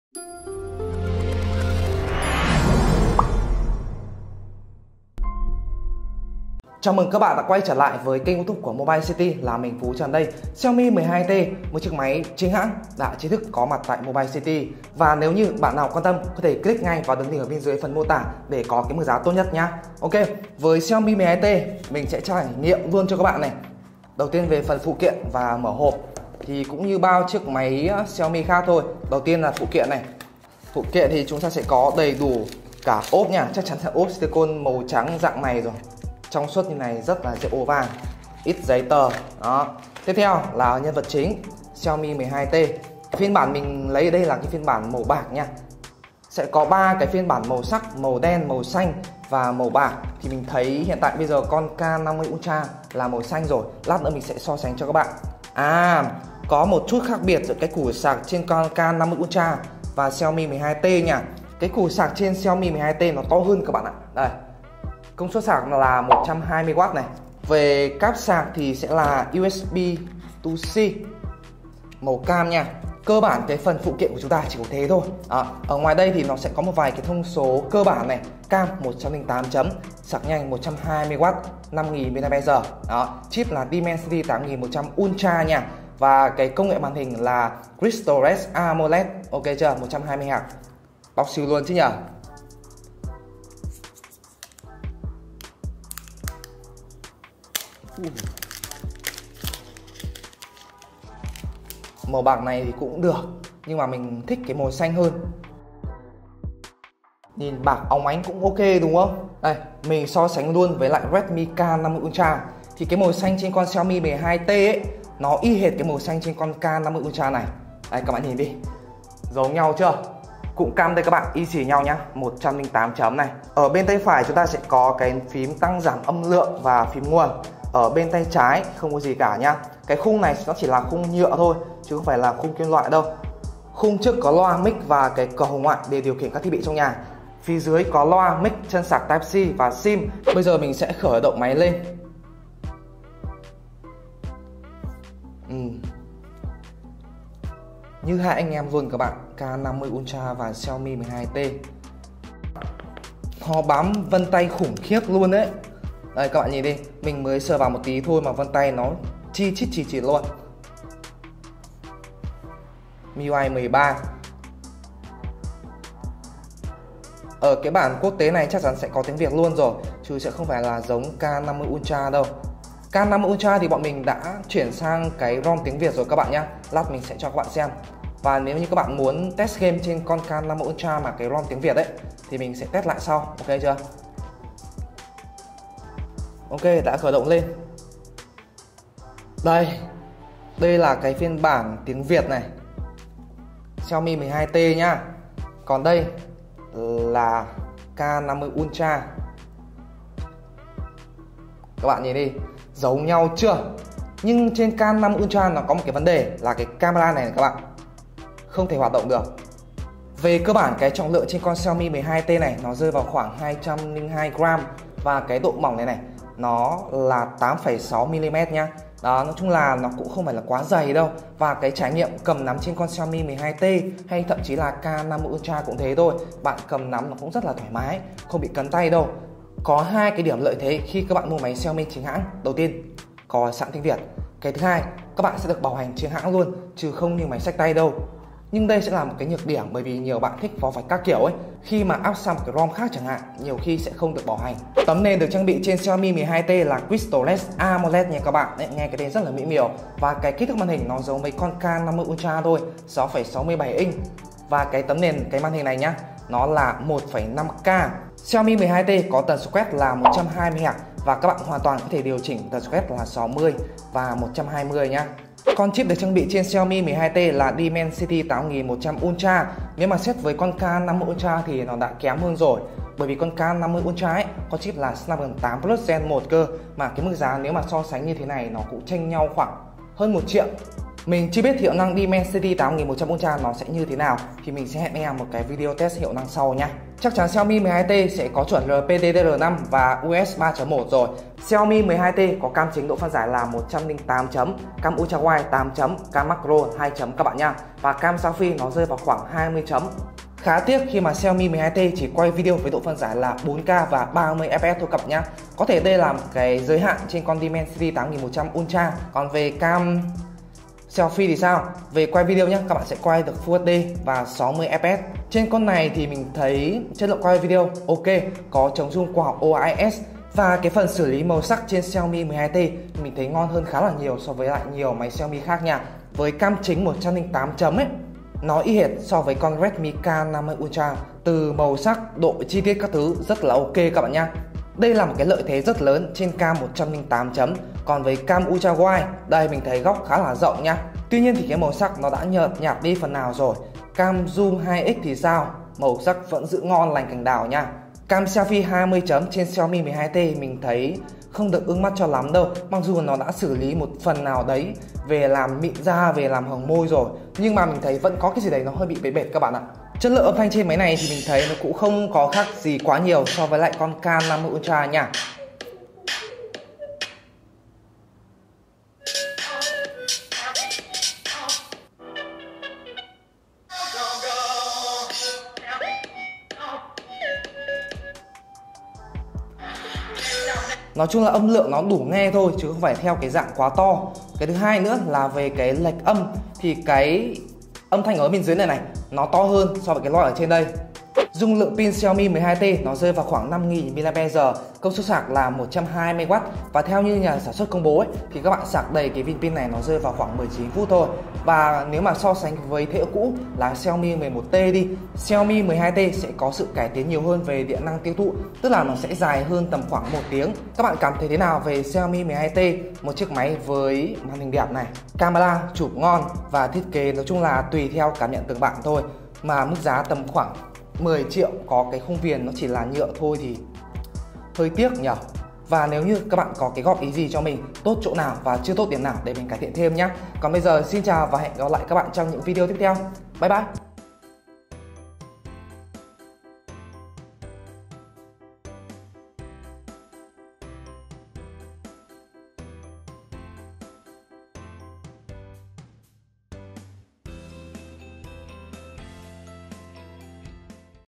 Mở hộp Xiaomi 12T Chính hãng: Tại sao không phải là Snapdragon 8+ Gen 1 :(((
Xiaomi 12T chính hãng sở hữu thiết kế và cấu hình phần cứng gần như giống với Xiaomi Redmi K50 Ultra, buồn thay chip trang bị đã giảm đáng kể. Xiaomi 12T có gì nổi bật?
 Xin mời các bạn xem video

0:00 Mở đầu
0:20 Phụ kiện
1:45 Thiết kế
4:04 Màn hình
4:25 Hiệu năng
4:51 Camera
5:59 Loa + Pin